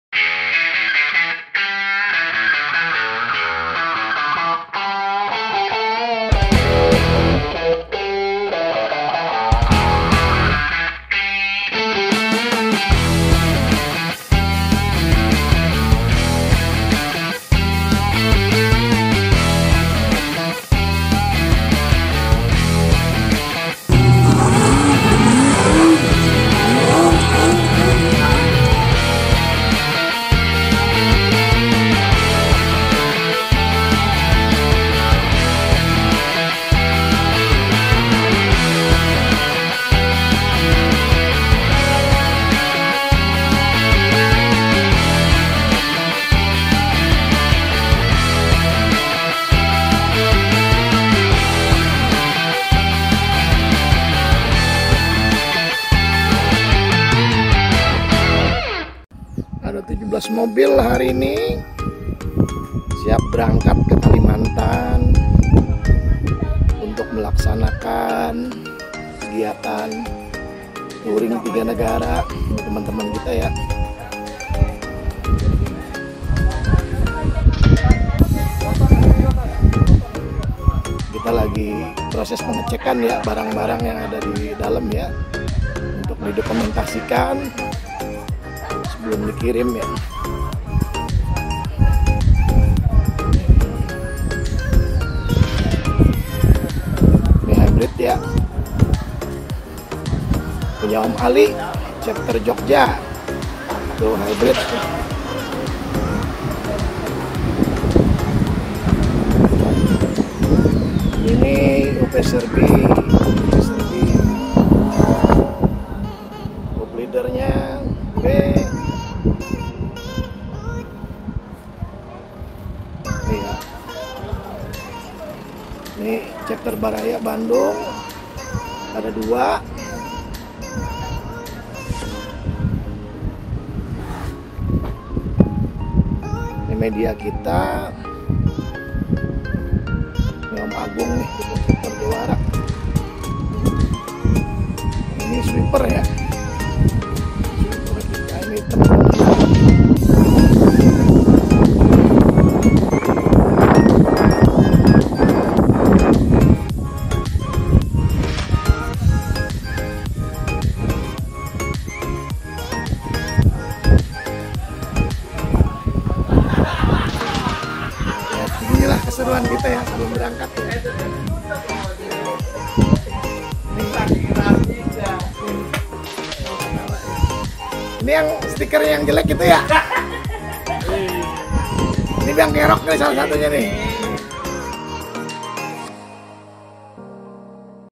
Road to WALI Touring 3 Negara - Explorer of Borneo: H -5
Hari Minggu, 17 Desember 2023 kemarin, para peserta Touring 3 Negara dari berbagai Chapter berkumpul untuk bersama-sama menuju Marunda Centre, Bekasi

Almaz para peserta touring dikumpulkan dan dipersiapkan  di sana untuk kemudian akan dikirim via kapal laut menuju Kota Pontianak. Sedangkan sebagian besar peserta akan menuju Pontianak pada tanggal 23 Desember via pesawat terbang

WALI ALMAZing All Around